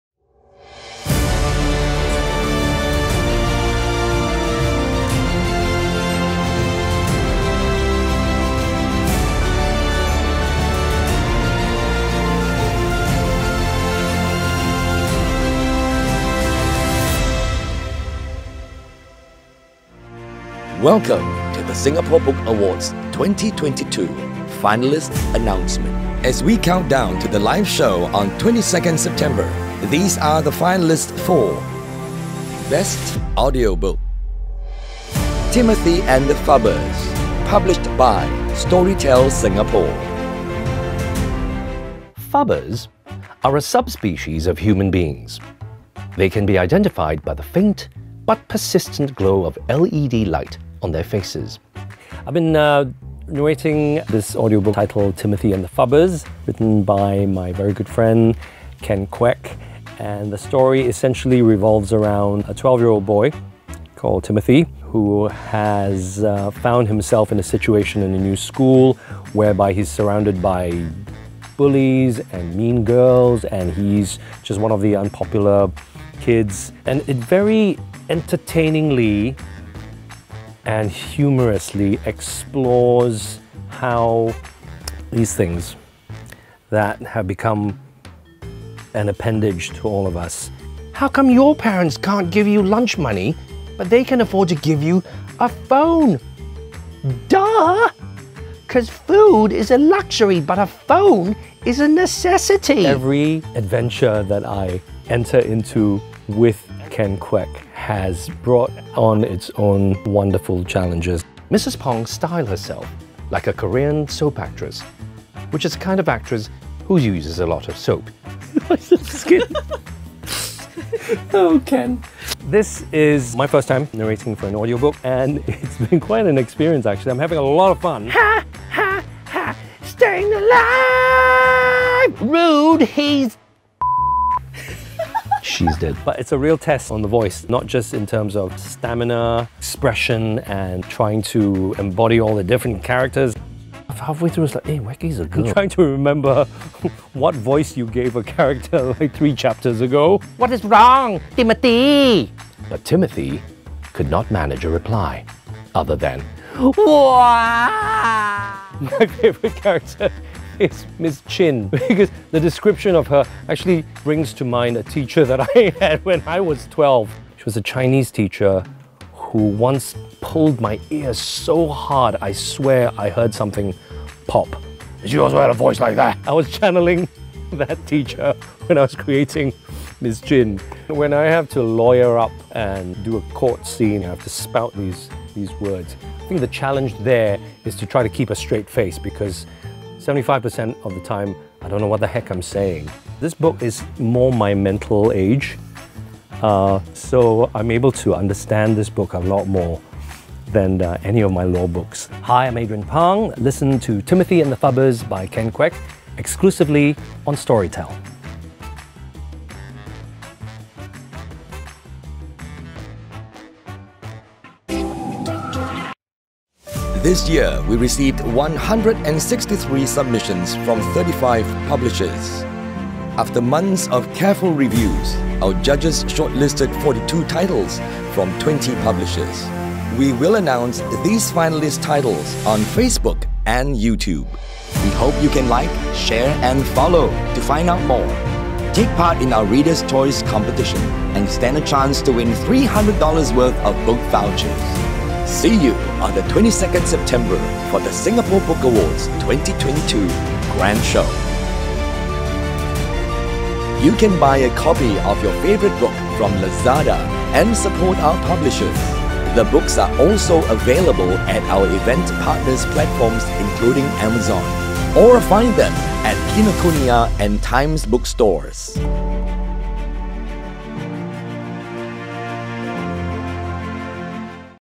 2022 SBA Finalist Showcase = Timothy and the Phubbers
Finalist for the Best Audiobook: Timothy and the Phubbers published by Storytel.